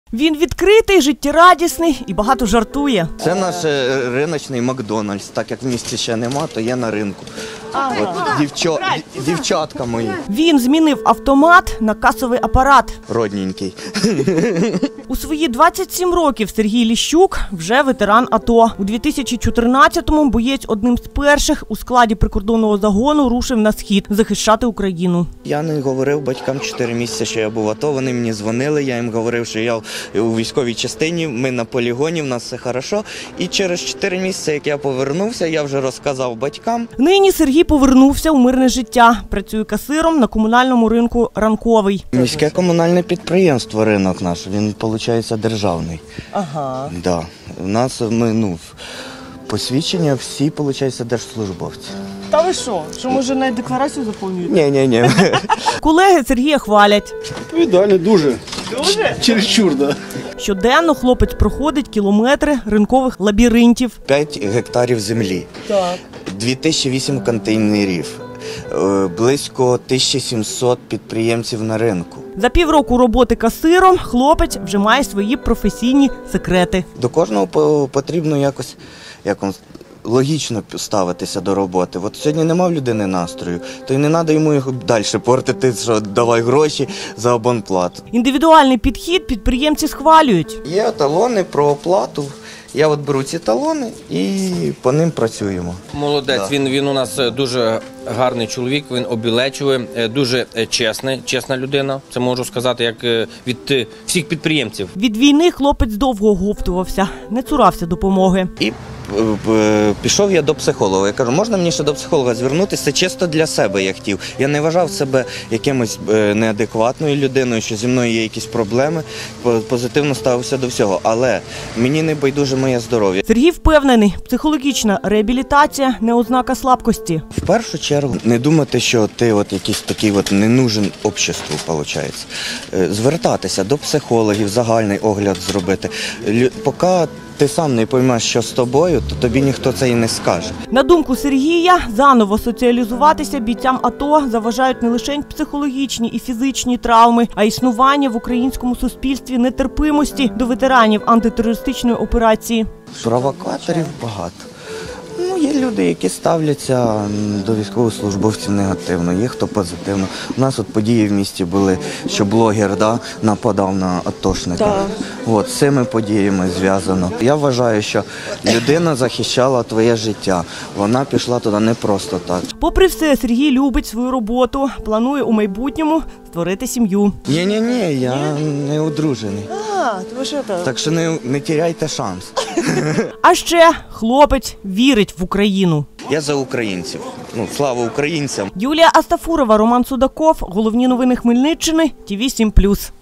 ТВ7+. Я_повертаюсь – хештег-акція підтримки для тих, хто воював.
ТВ7+. Я_повертаюсь – хештег-акція, що доводить - ті, хто пройшов війну, можуть бути успішними в мирі.